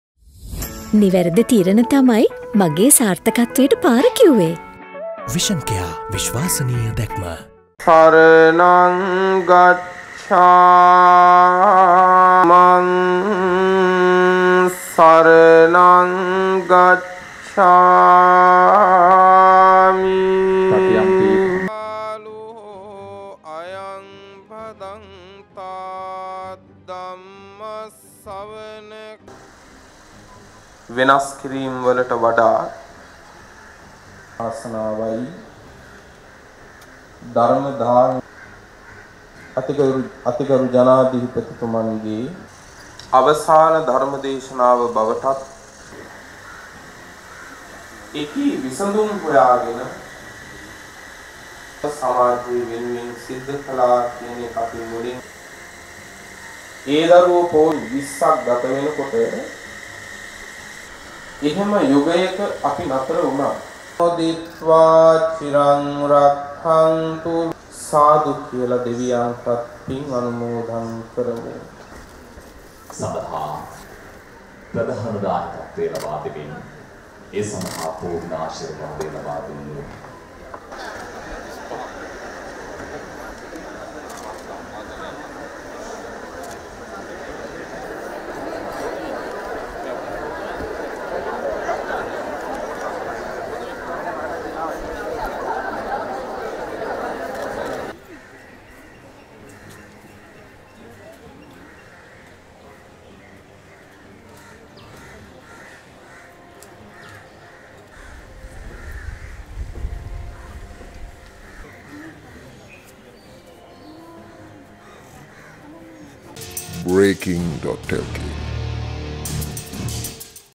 ධුරයෙන් සමුගැනීමට පෙර ජනාධිපතිතුමා සහභාගී වන අවසන් ධර්ම දේශනාව | Breaking.lk
ධුරයෙන් සමුගැනීමට පෙර ජනාධිපතිතුමා සහභාගී වන අවසන් ධර්ම දේශනාව